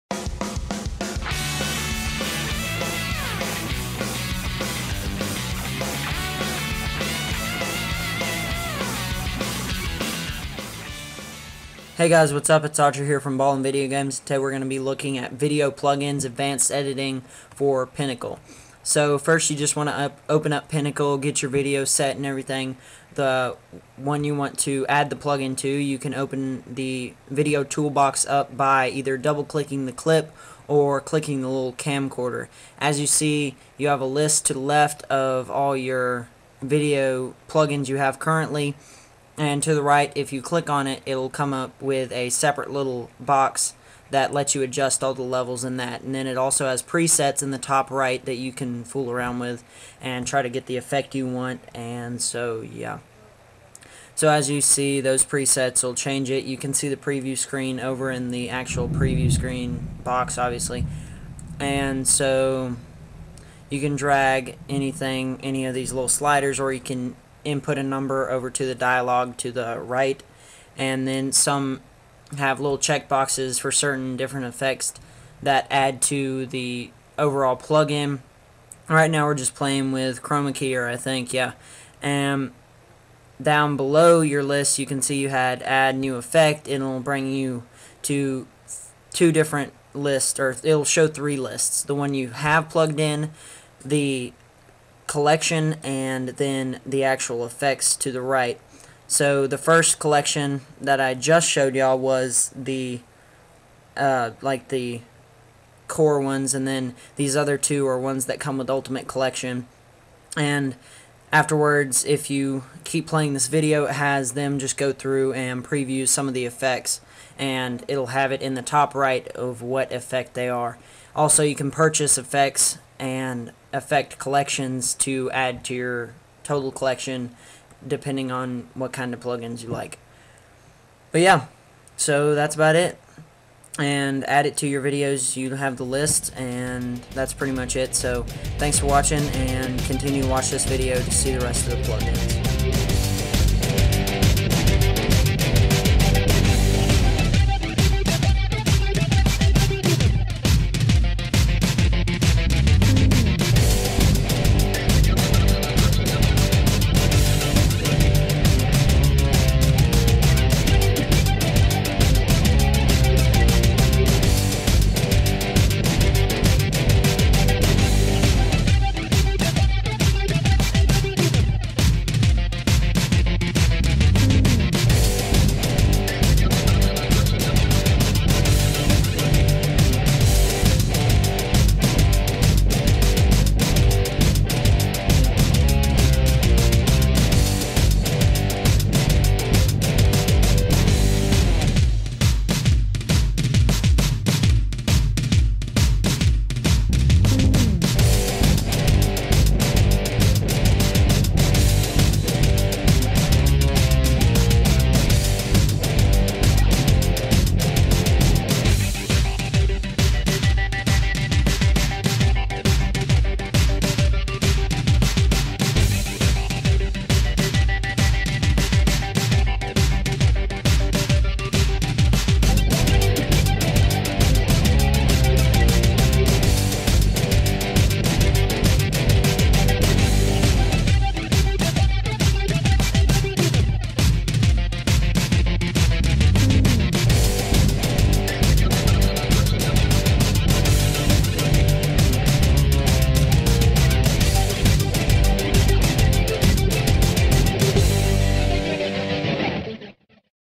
Pinnacle Studio: Video Plugins Tutorial
We show you how to add video plugins to your video in Pinnacle Studio 14.